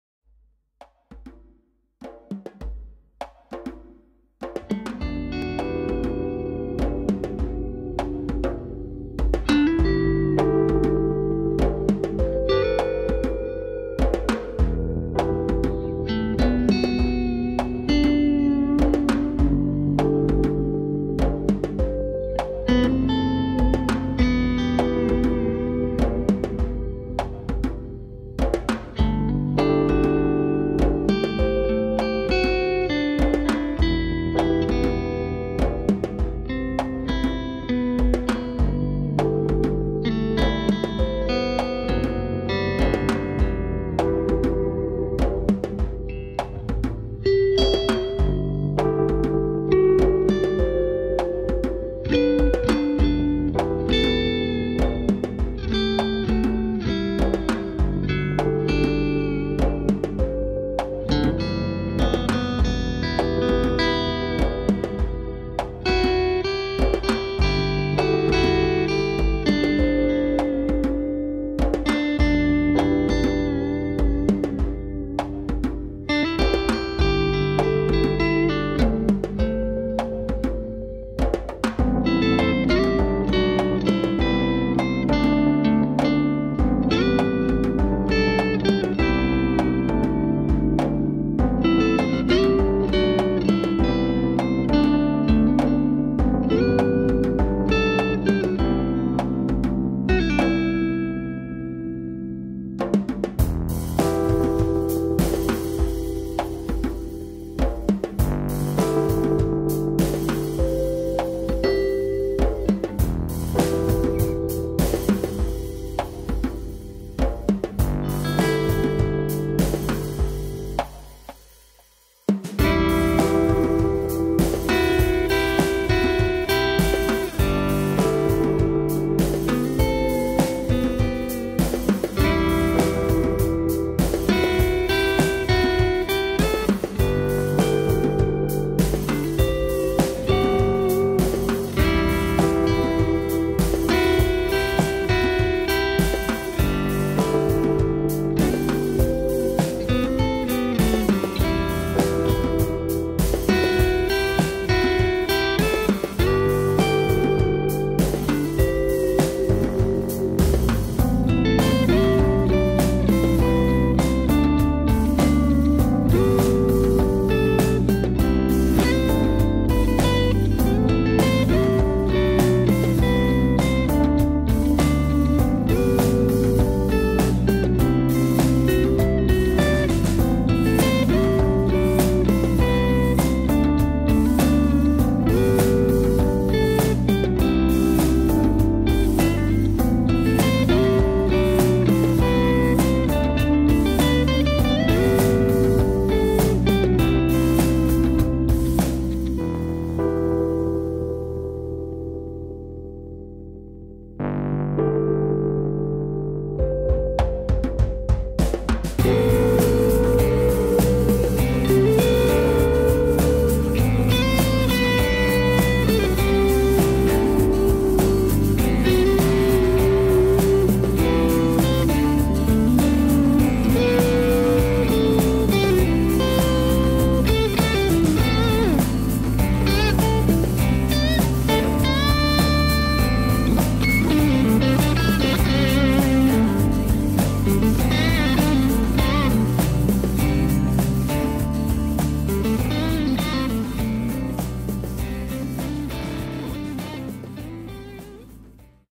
Hot air balloon flight Carmarthenshire July 2014
Flying in the Towy Valley west of Dryslwyn just after sunrise, one hour of flight in four minutes!